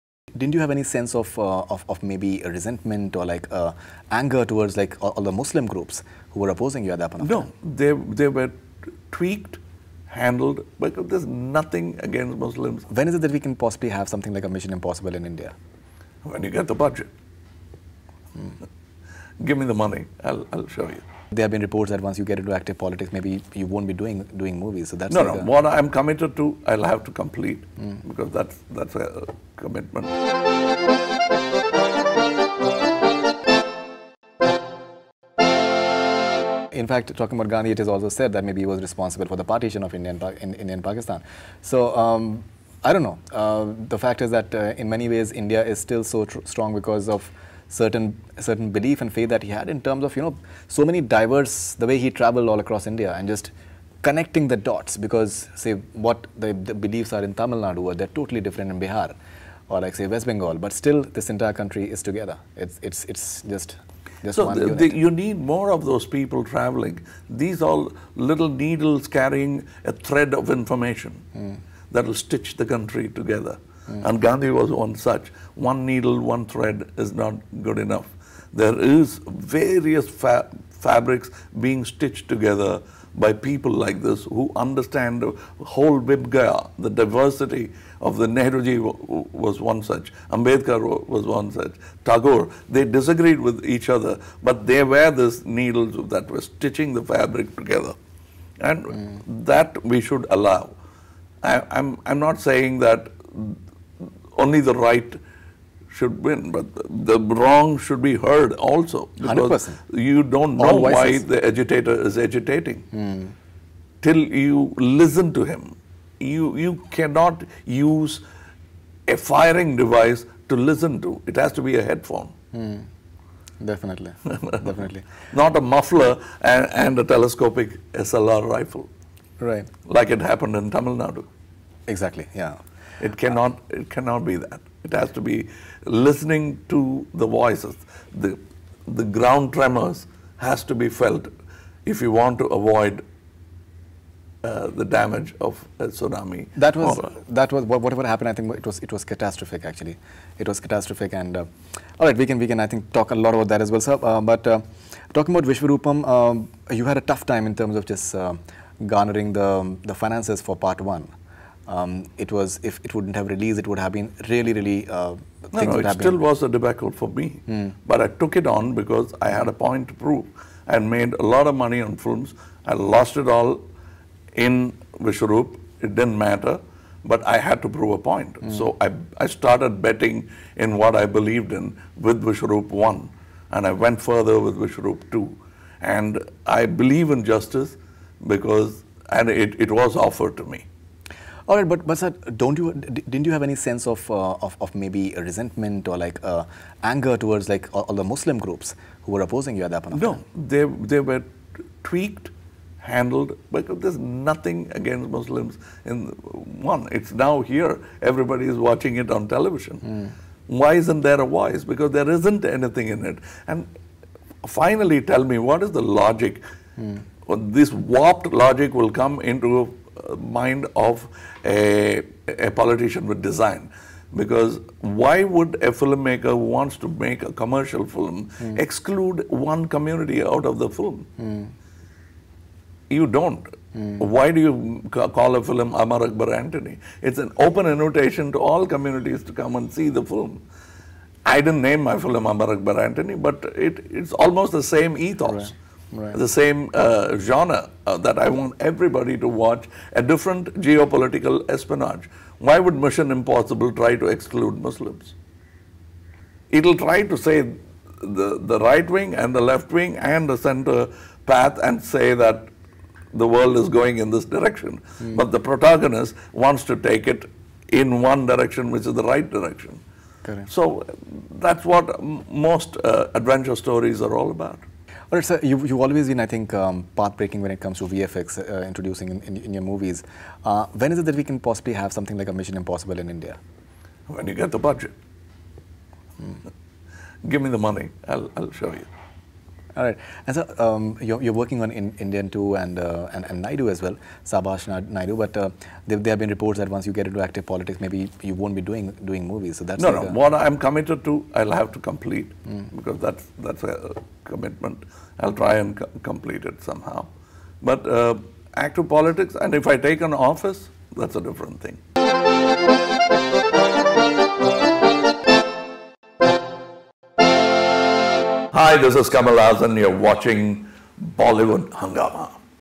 "We shall have a film like Mission Impossible in India when...": Kamal Haasan
Kamal Haasan in part 5 of his Bollywood Hungama exclusive interview talks about why he didn't have any animosity towards the Muslim groups who opposed Vishwaroopam. He also talks about a film like Mission Impossible and when we in India can possibly make a film of that magnitude. Must watch!